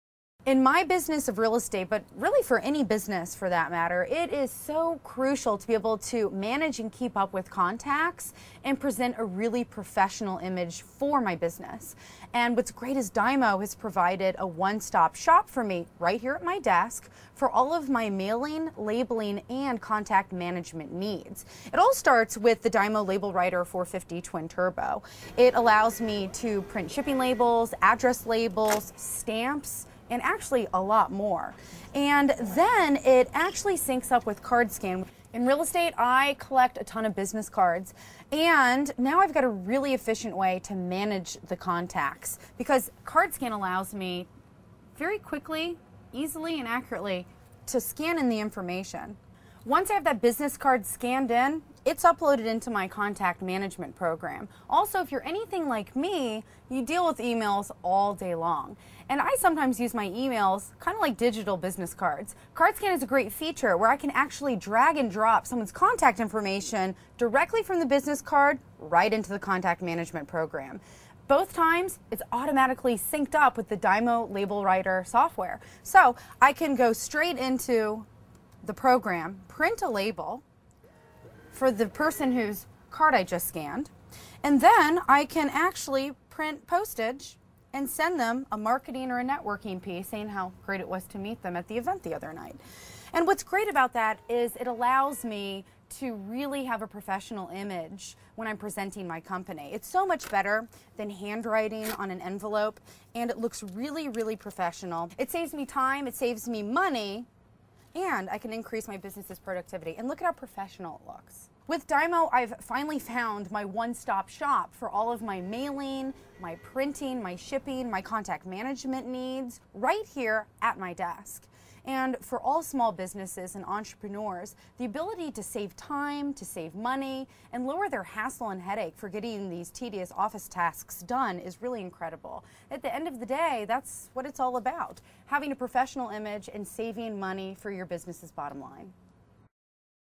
DYMO LabelWriter
Connect the DYMO LabelWriter label printer to your PC or Mac®, and you're ready to print impressive address labels, shipping labels, file folder labels, name badges, inventory labels, barcode labels and more -- all without the extra work and hassles of printing sheet labels on a standard desktop printer.